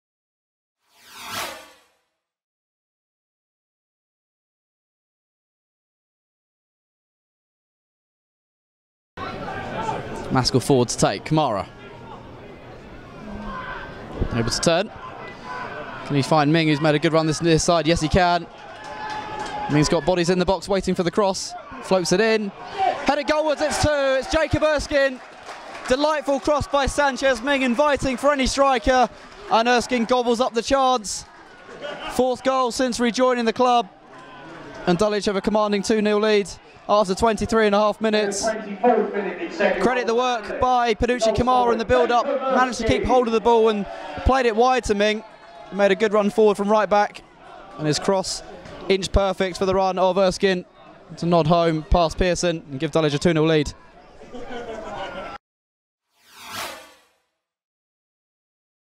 Jacob Erskine vs Tooting & Mitcham United, London Senior Cup Quarter-Final, 06/04/17
Tooting & Mitcham had seen a header cleared off the line and a free-kick whistle wide since falling behind, bu Dulwich would double their advantage as Panutche Camara danced past a couple of challenges before feeding Sanchez Ming out wide. The right-back produced an inch-perfect cross for Jacob Erskine to run on to, and he planted his header past Matte Pierson. Dulwich 2-0 Tooting & Mitcham United.